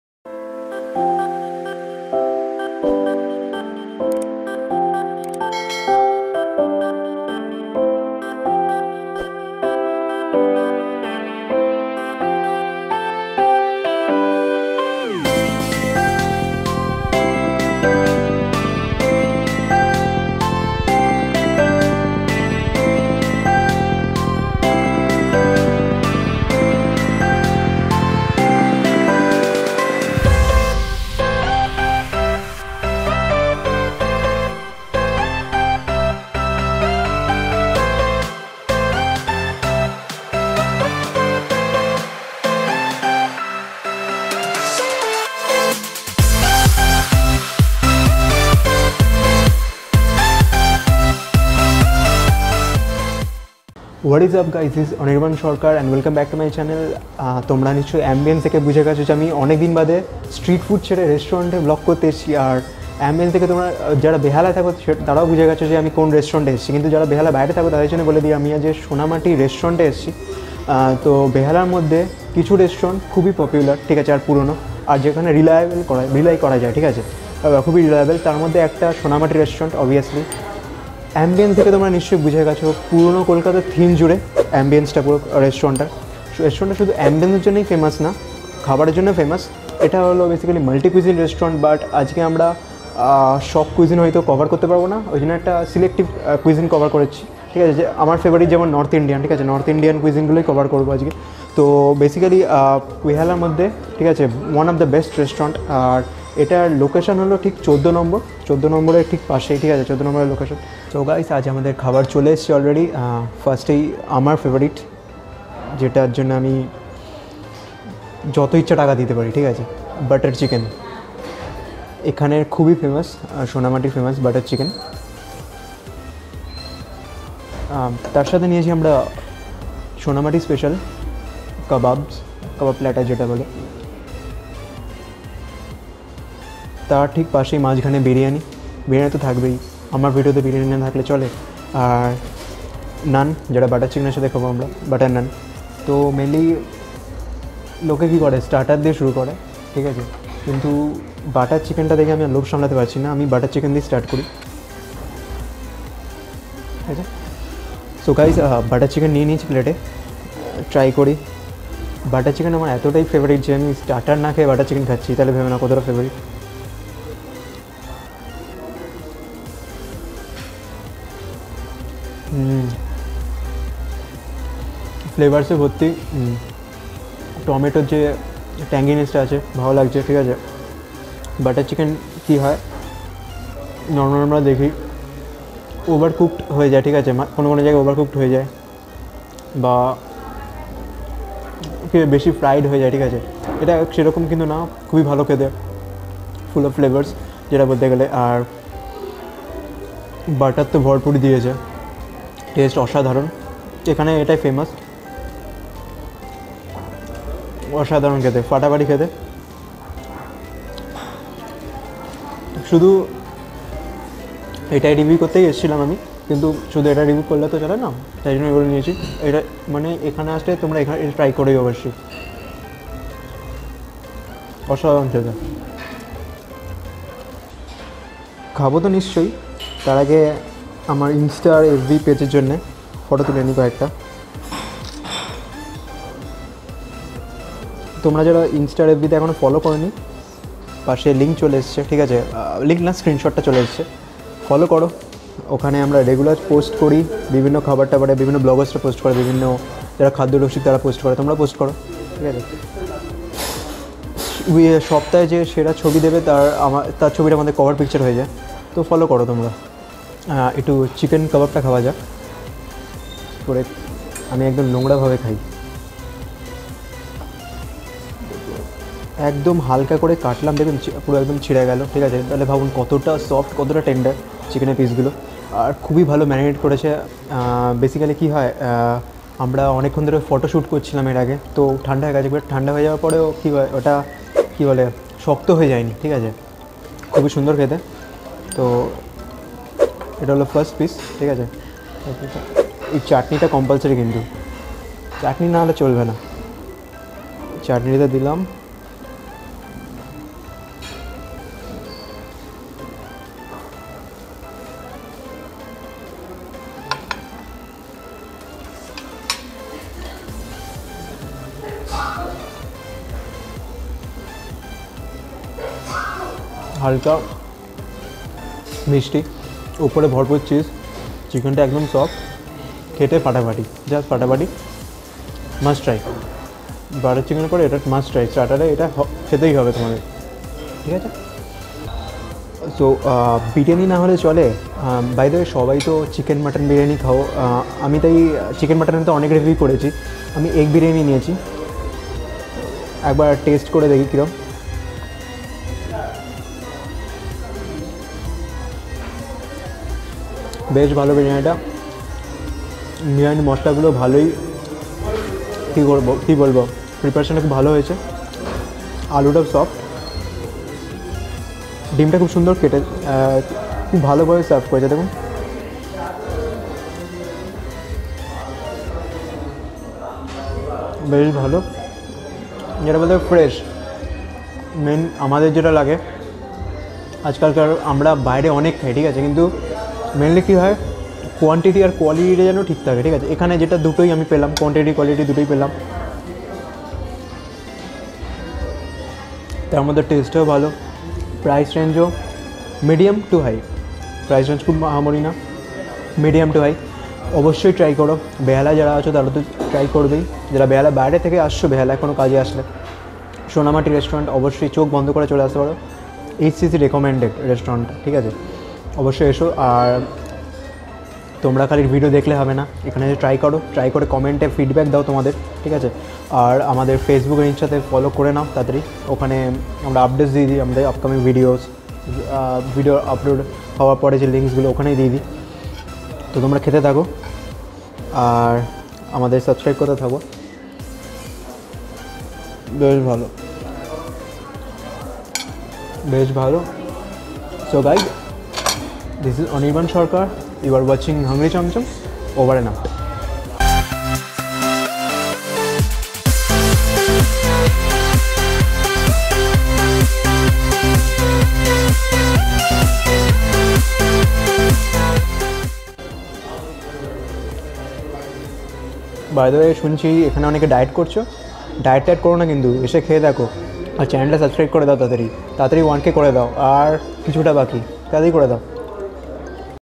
🔥BUTTER CHICKEN |Multicuisine Restaurant In Kolkata |Sonamati Restaurant | কলকাতা ফুড ব্লগ
Sonamati Restaurnt is one of the best restaurants in behala, we have tried some of their signature dishes .
Behalai Ei Restaurant Obosshoi Try koro.

location Link 👉Sonamati Restaurant

282, Diamond Harbour Rd, Mukund Das Colony, Behala, Kolkata, West Bengal 700034